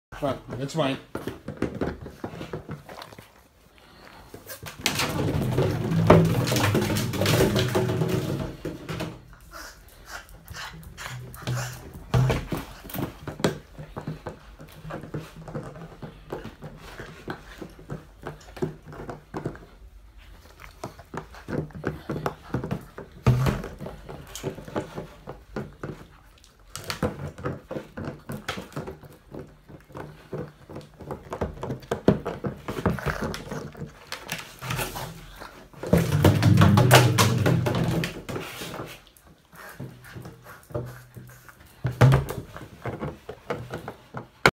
Size does not matter ::)
Lola the Boston terrier playing with he water jug